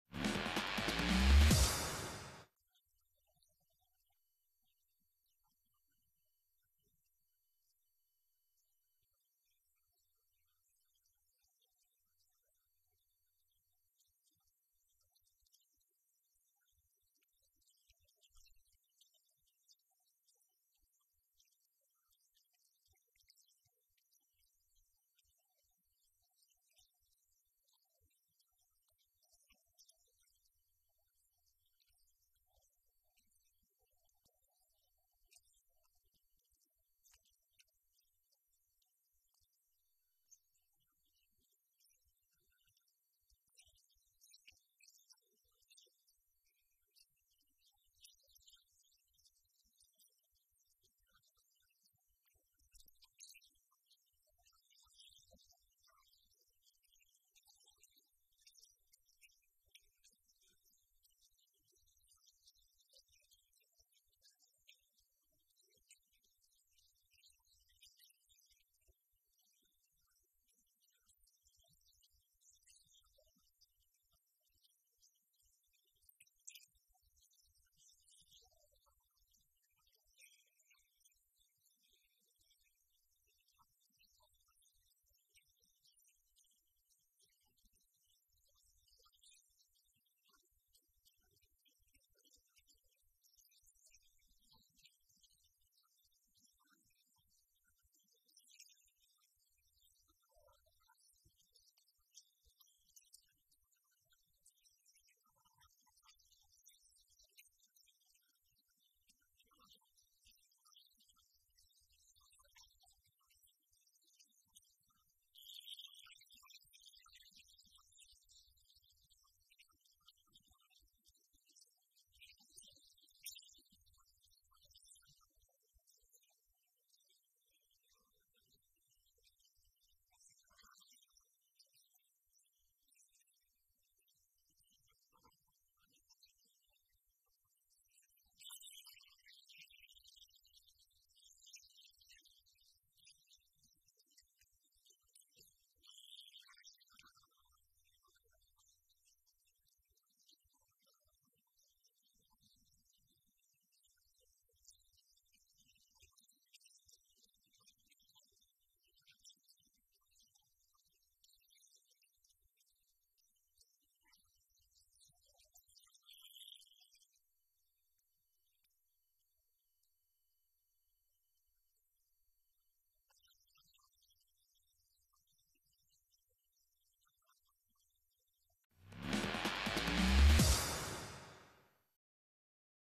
ABA League 2 2021/22 highlights, Round 7: Mladost MaxBet - Rogaška (19.12.2021)
Take a look at the highlights of the 2021/22 season ABA League 2 game between Mladost MaxBet and Rogaška in Hills Arena, Sarajevo.